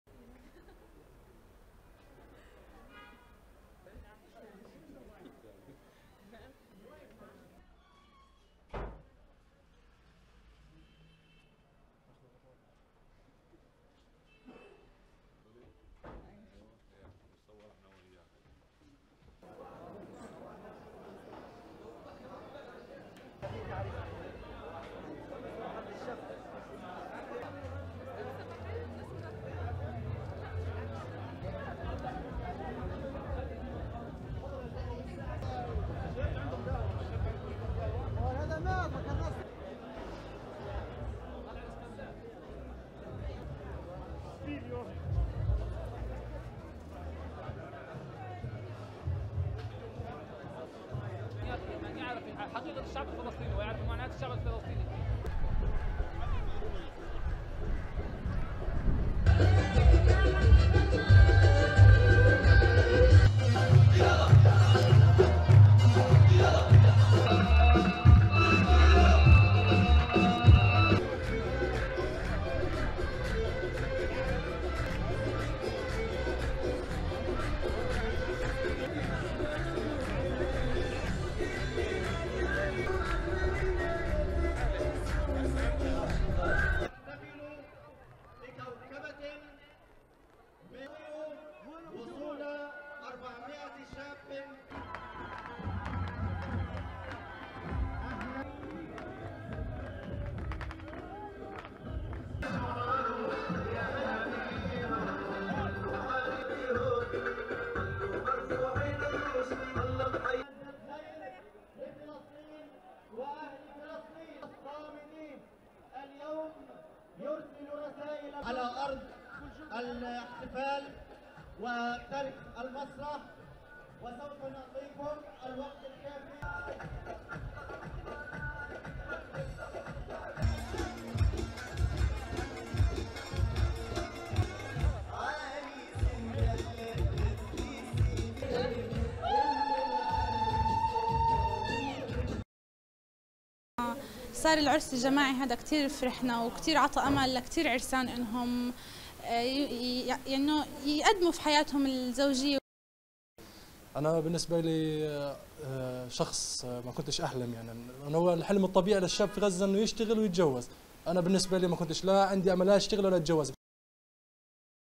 Mass wedding ceremony held in Gaza City
Nearly 200 Palestinian couples were married in a ceremony funded by an Emirati governmental organisation, during a mass wedding ceremony in Gaza City on Saturday.

The event on is aimed at helping the underprivileged, war casualties and those whose houses were destroyed in the recent Israeli war on Gaza, state news agency Wam reported. The organising committee of the mass wedding said that each of the bridegrooms will receive US$4,000 to help them establish a new family.

“The celebration will see surprises and programmes that will make the Gazans happy because Gaza deserves more happiness, freedom and peace,” the committee said.

The event was organised under the patronage of Sheikh Mansour bin Zayed, Deputy Prime Minister and Minister of Presidential Affairs.

Sara Zohod, Bride: "This mass wedding made us really happy and gave hope for other young couples."

Islam Barakat, Bridegroom: "I never dreamed in my life of doing that. The guys in Gaza work hard to get married. Honestly, I had no job so I thought I wasn't able to take this step."